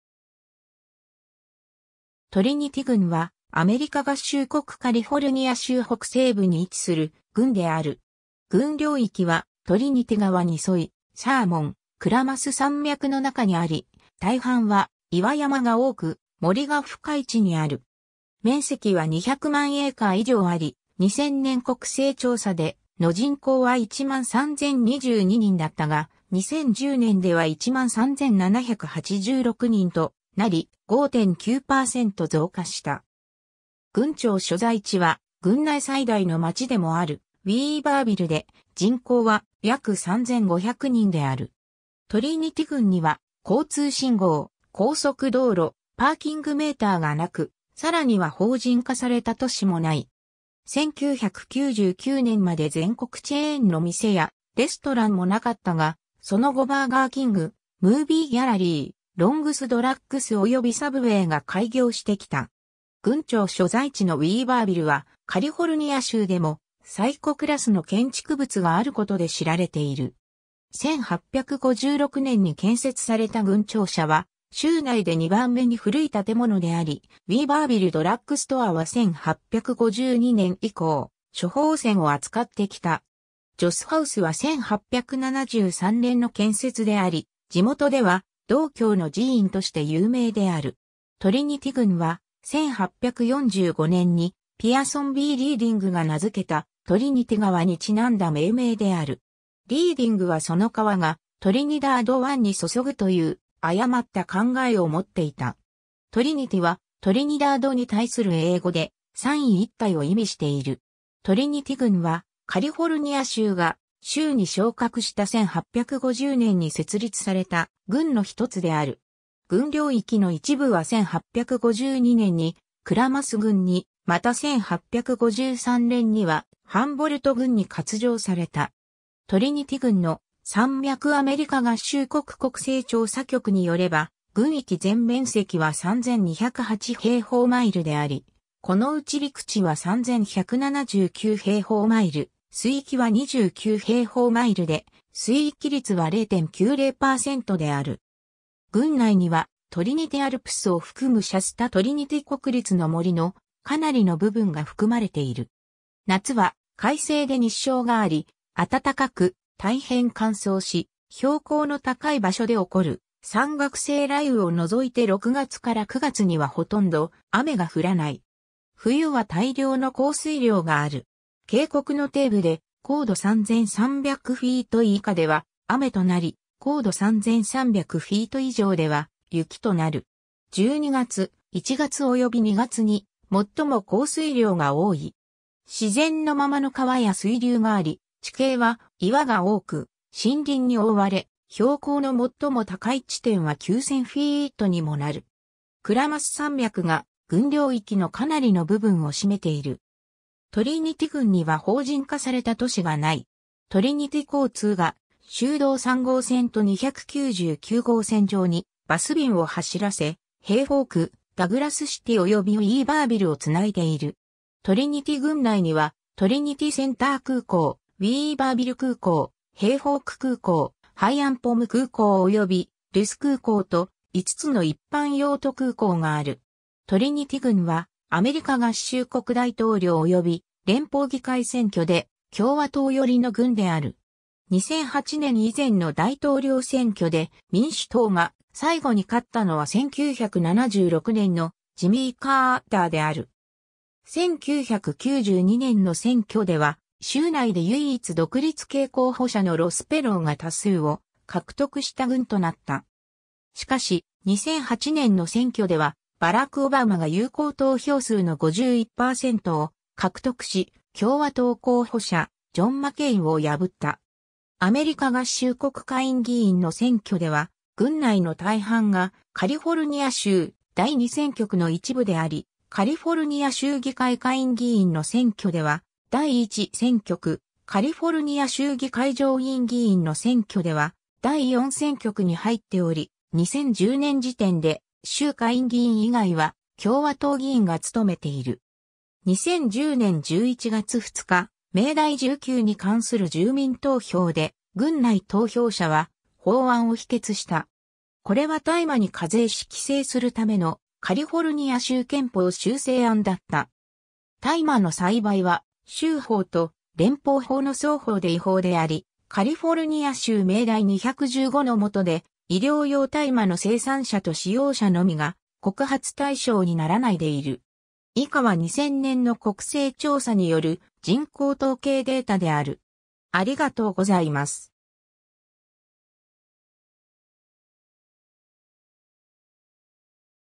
トリニティ郡 (カリフォルニア州)
トリニティ郡（英: Trinity County）は、アメリカ合衆国カリフォルニア州北西部に位置する郡である。
郡領域はトリニティ川に沿い、サーモン/クラマス山脈の中にあり、大半は岩山が多く森が深い地にある。
面積は200万エーカー (8,000 km2) 以上あり、2000年国勢調査での人口は13,022人だったが、2010年では13,786人となり、5.9%増加した。
郡庁所在地は郡内最大の町でもあるウィーバービルで、人口は約3,500人である。
 トリニティ郡には交通信号、高速道路、パーキングメーターがなく、さらには法人化された都市も無い。
1999年まで全国チェーンの店やレストランも無かったが、その後バーガーキング、ムービーギャラリー（その後閉鎖）、ロングスドラッグス（現在はCVS/ファーマシー）およびサブウェイが開業してきた。
 郡庁所在地のウィーバービルはカリフォルニア州でも最古クラスの建築物があることで知られている。
1856年に建設された郡庁舎は州内で2番目に古い建物であり、ウィーバービル・ドラッグストアは1852年以降処方箋を扱ってきた。
ジョス・ハウスは1873年の建設であり地元では道教の寺院として有名である。
 トリニティ郡は、1845年にピアソン・B・リーディングが名付けたトリニティ川に因んだ命名である。
リーディングはその川がトリニダード湾に注ぐという誤った考えを持っていた。
トリニティはトリニダードに対する英語で、三位一体を意味している。
 トリニティ郡はカリフォルニア州が州に昇格した1850年に設立された郡の1つである。
郡領域の一部は1852年にクラマス郡（現在は廃止）に、また1853年にはハンボルト郡に割譲された。
 トリニティ郡の山脈 アメリカ合衆国国勢調査局に拠れば、郡域全面積は3,208平方マイル (8,307 km2)であり、このうち陸地は3,179平方マイル (8,233 km2)、水域は29平方マイル (75 km2)で水域率は0.90%である。
郡内にはトリニティ・アルプスを含むシャスタ・トリニティ国立の森のかなりの部分が含まれている。
夏は快晴で日照があり、暖かく、大変乾燥し、標高の高い場所で起こる山岳性雷雨を除いて6月から9月にはほとんど雨が降らない。
冬は大量の降水量がある。
渓谷の底部で高度3,300フィート (1,000 m) 以下では雨となり、高度3,300 フィート (1,000 m) 以上では雪となる。
12月、1月および2月に最も降水量が多い。
自然のままの川や水流があり、地形は岩が多く森林に覆われ、標高の最も高い地点は 9,000フィート (2,700 m) にもなる。
クラマス山脈が郡領域のかなりの部分を占めている。
 トリニティ郡には法人化された都市が無い。
 トリニティ交通が州道3号線と299号線上にバス便を走らせ、ヘイフォーク、ダグラスシティおよびウィーバービルを繋いでいる。
 トリニティ郡内にはトリニティセンター空港、ウィーバービル空港、ヘイフォーク空港、ハイアンポム空港およびルス空港と、5つの一般用途空港がある。
 トリニティ郡はアメリカ合衆国大統領および連邦議会選挙で共和党寄りの郡である。
2008年以前の大統領選挙で民主党が最後に勝ったのは1976年のジミー・カーターである。
1992年の選挙では州内で唯一独立系候補者のロス・ペローが多数を獲得した郡となった。
しかし、2008年の選挙ではバラク・オバマが有効投票数の51%を獲得し、共和党候補者ジョン・マケイン（46%）を破った。
 アメリカ合衆国下院議員の選挙では郡内の大半がカリフォルニア州第2選挙区の一部であり、カリフォルニア州議会下院議員の選挙では第1選挙区、カリフォルニア州議会上院議員の選挙では第4選挙区に入っており、2010年時点で州下院議員以外は共和党議員が努めている。
 2010年11月2日、命題19に関する住民投票で郡内投票者は法案を否決した。
これは大麻に課税し規制するためのカリフォルニア州憲法修正案だった。
大麻の栽培は州法と連邦法の双方で違法であり、カリフォルニア州命題215の元で、医療用大麻の生産者と使用者のみが告発対象にならないでいる。